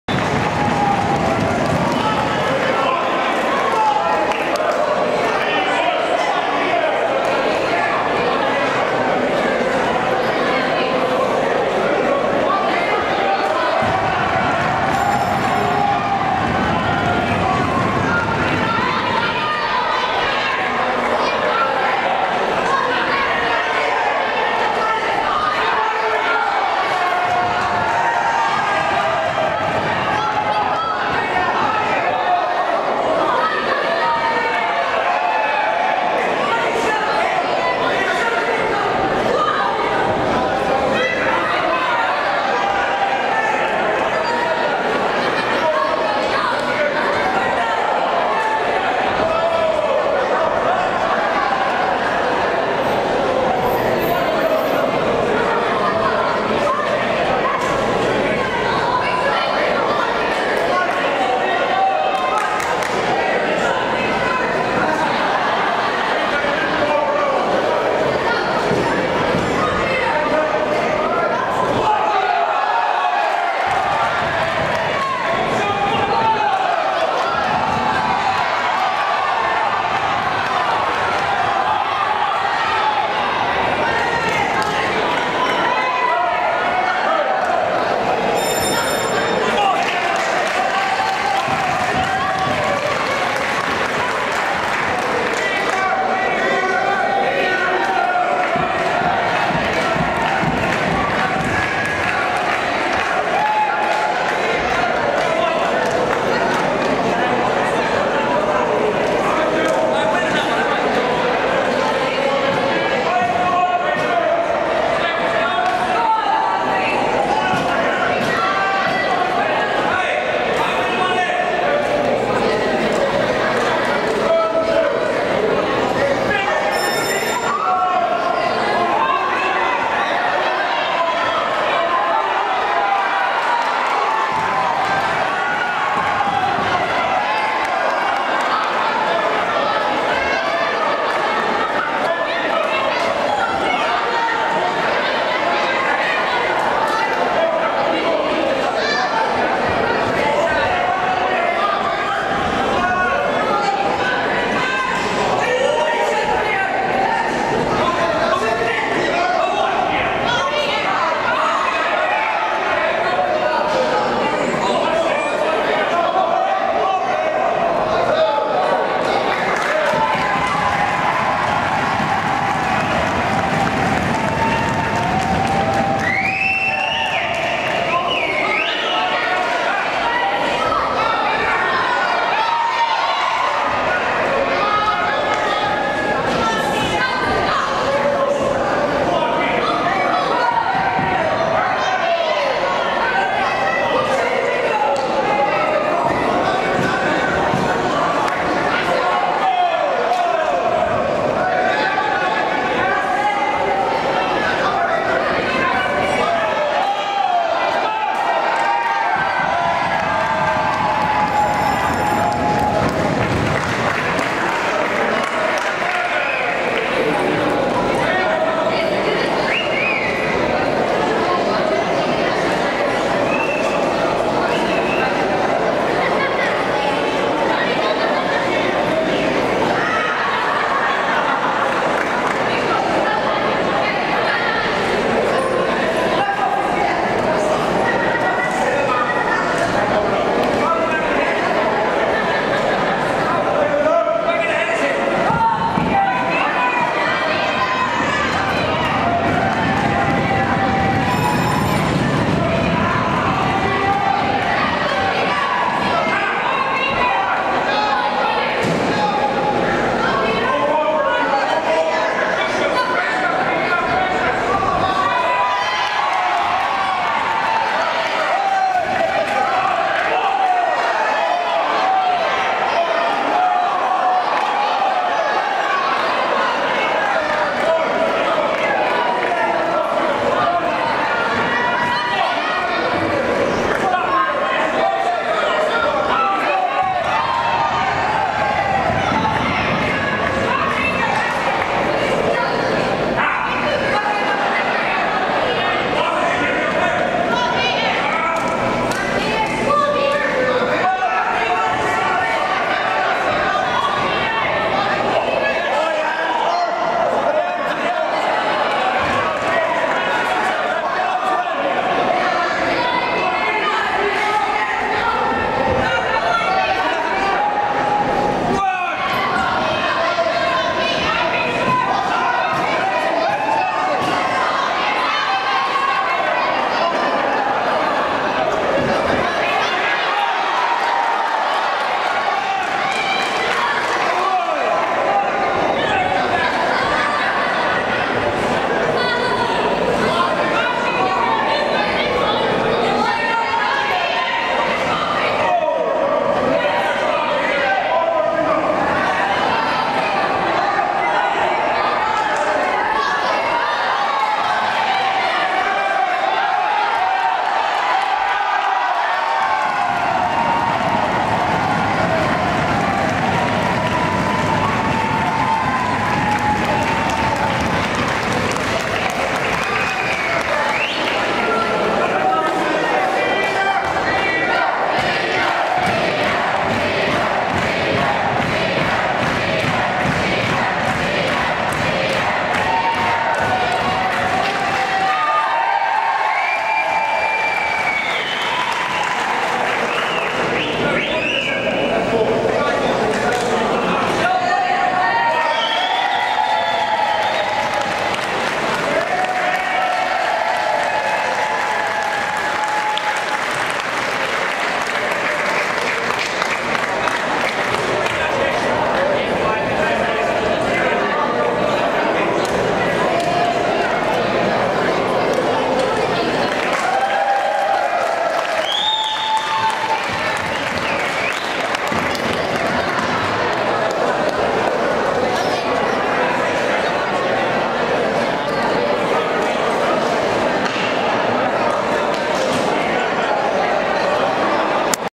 Wexford Box Cup 2022
Wexford Box Cup 2022 Fight Between Olympic Boxer Johnny Joyce and Peter Griffiths Baker ( The Red Rocket )